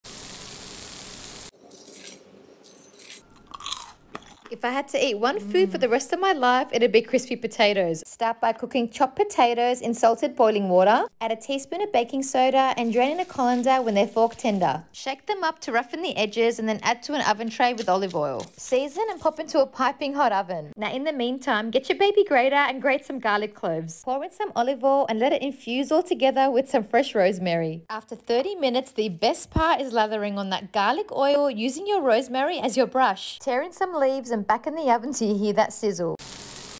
Crispy Roasted Potatoes
These Crispy Roasted Potatoes make the perfect side dish. They’re topped with an infused garlic rosemary olive oil that tastes heavenly